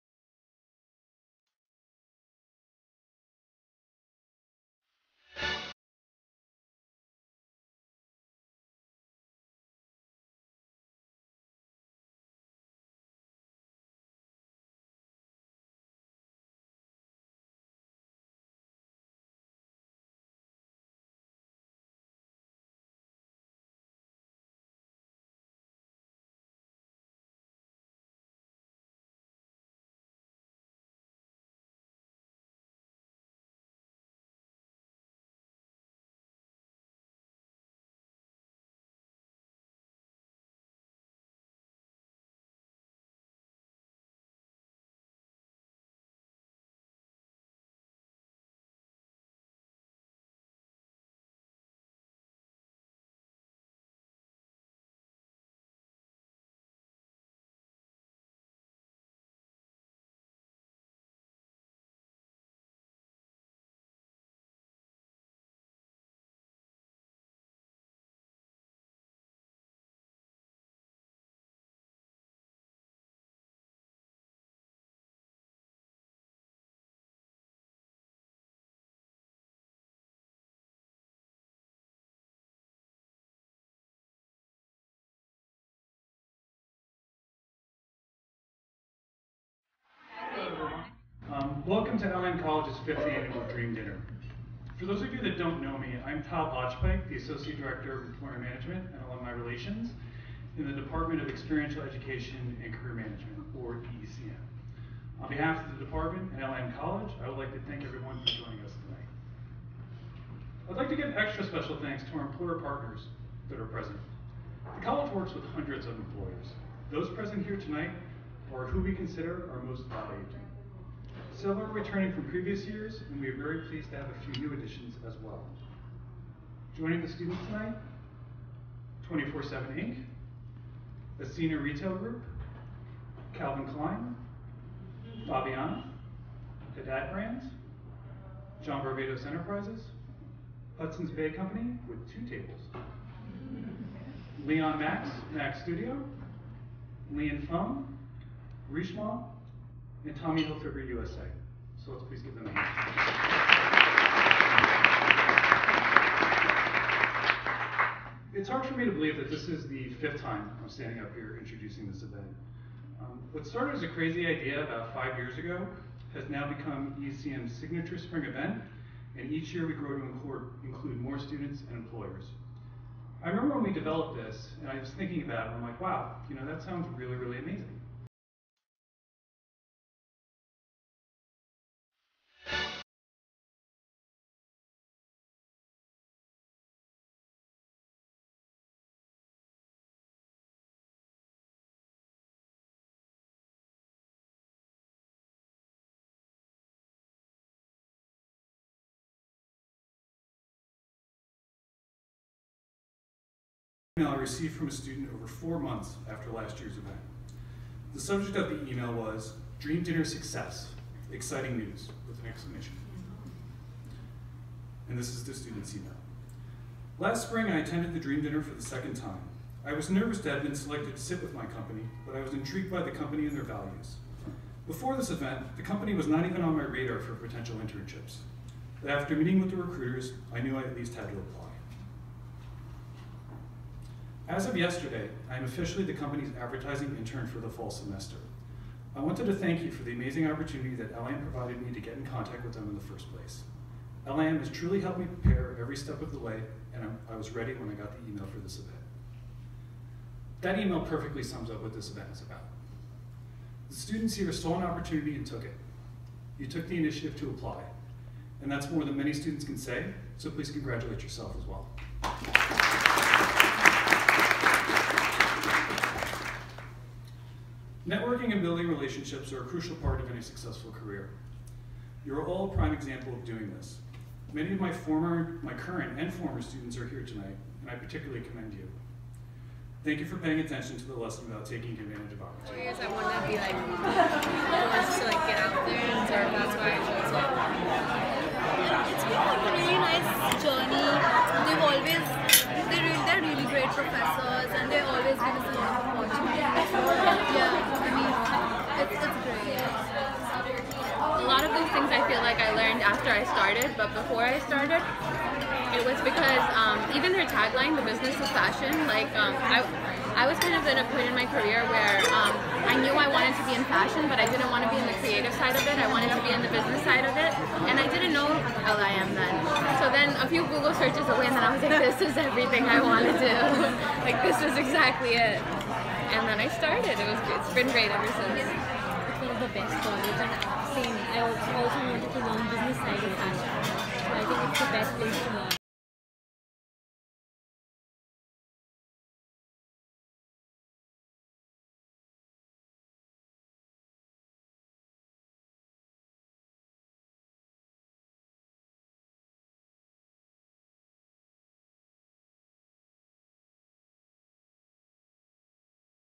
Living the Dream at LIM Dream Dinner
LIM College’s annual Dream Dinner offers a select group of LIM College students an exclusive evening of networking and career exploration, featuring representatives from several leading fashion companies. LIM College's Department of Experiential Education & Career Management present this annual Dream Dinner Event.

Special thanks to the participating companies in the 2018 Dream Dinner: Ascena Retail Group, Calvin Klein, Faviana, Haddad Brands, Hudson’s Bay Company, John Varvatos Enterprises, Leon Max Inc., Li & Fung, Richemont, Tommy Hilfiger, and 24 Seven.

Interested in applying or learning more about LIM College?